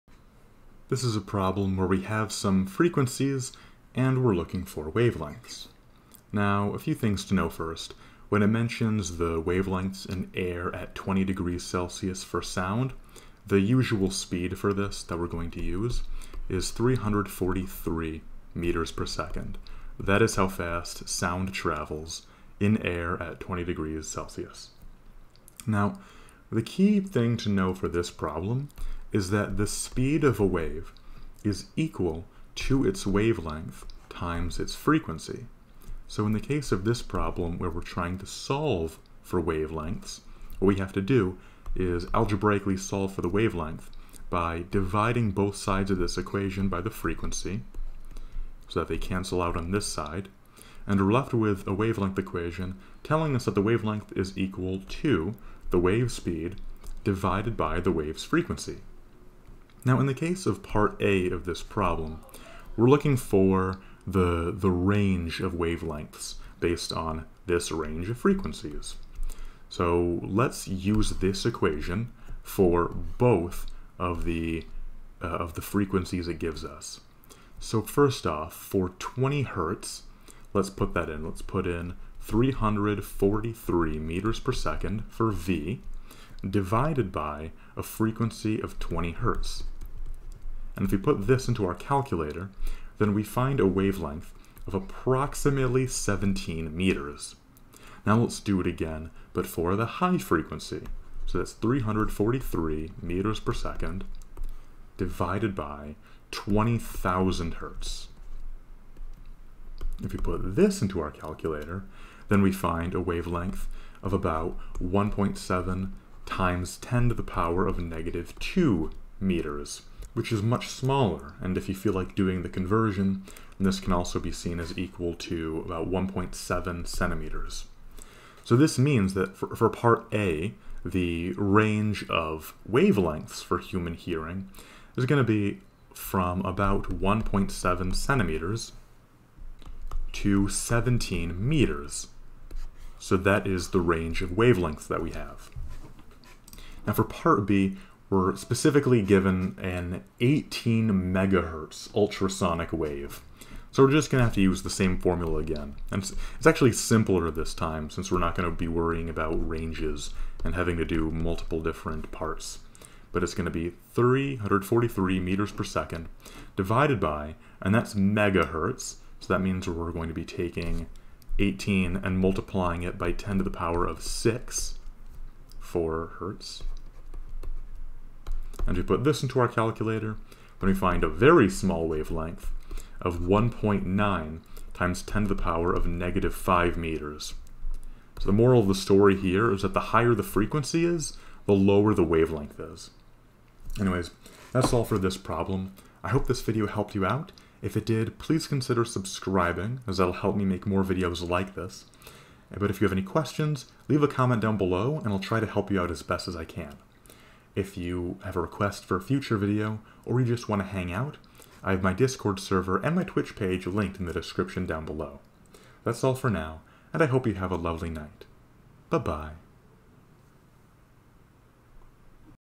(a) Calculate the wavelengths in air at 20°C for sounds in the maximum range of human hearing, 20 Hz
(a) Calculate the wavelengths in air at 20°C for sounds in the maximum range of human hearing, 20 Hz to 20,000 Hz. (b) What is the wavelength of an 18-MHz ultrasonic wave?